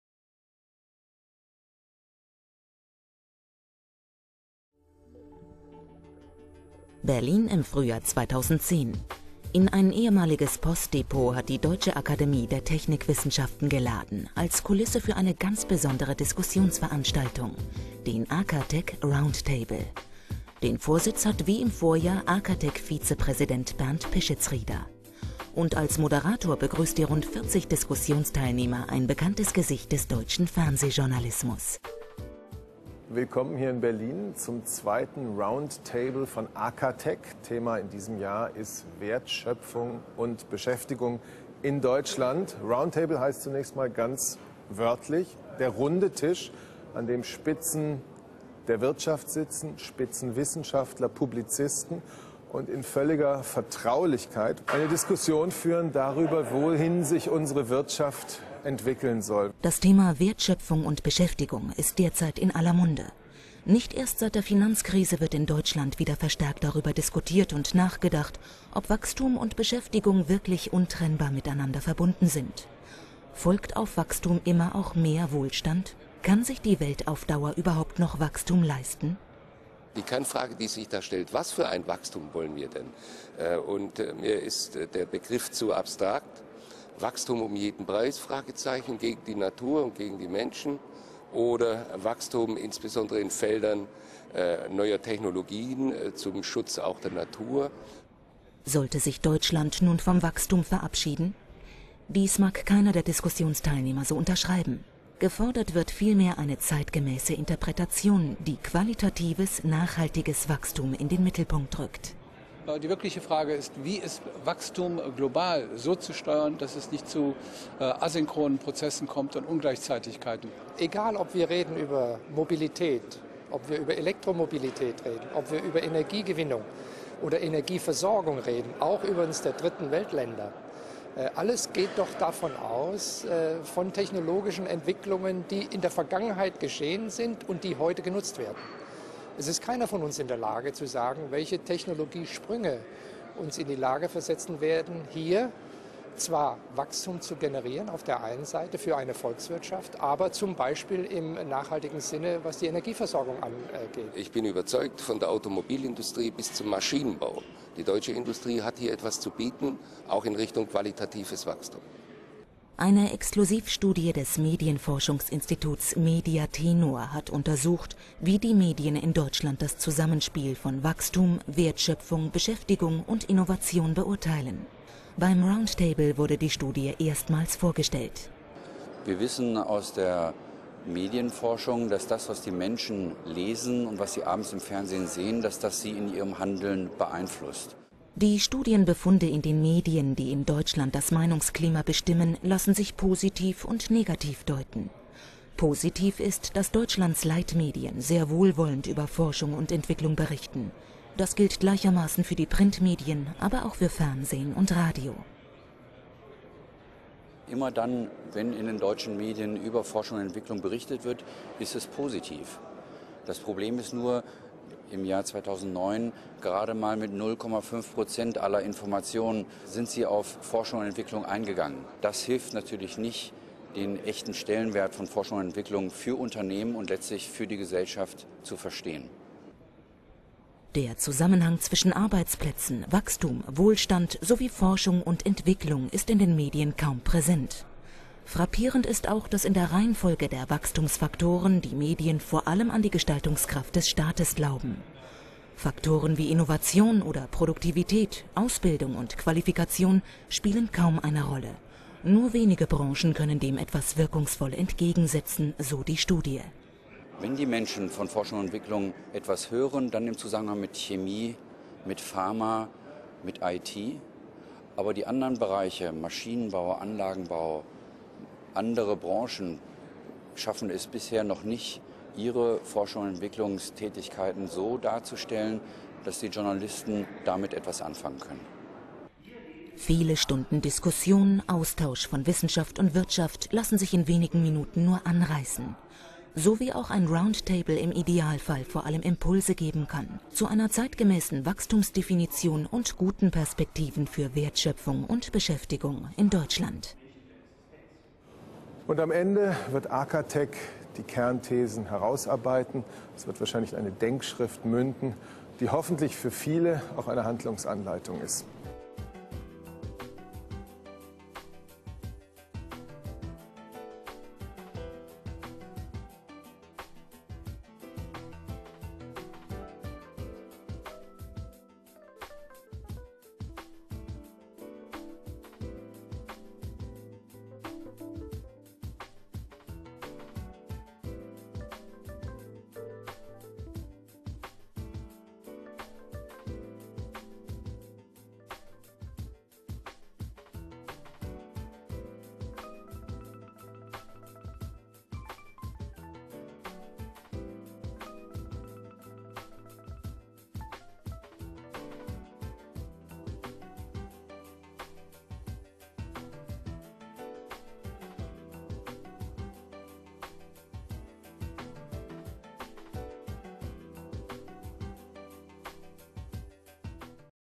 Stimmen und Eindrücke vom Round Table
Beim Round Table diskutieren hochkarätige Wissenschaftler, Wirtschaftsvertreter und Publizisten moderiert von Steffen Seibert wichtige Zukunftsfragen, die gerade in Krisenjahren in den Hintergrund geraten können. Ging es 2009 um den Nachwuchsmangel in technischen Berufen, der Deutschland langfristiges Wachstum kosten könnte, stand 2010 der Wachstumsbegriff selbst im Zentrum. Angesichts der Wirtschaftskrise und der sich verschärfenden Klimaproblematik wurde Wirtschaftswachstum als übergeordnetes Ziel insbesondere in den Medien in Frage gestellt. Ist Wirtschaftswachstum möglich, das nicht zwangläufig zu einem steigenden Ressourcenverbrauch führt? Das Video zeigt Eindrücke aus der Veranstaltung am 1. März 2010 in der Station Berlin und Statements von Teilnehmern aus Wissenschaft, Publizistik, Wirtschaft und Gewerkschaften am Rande des Round Table.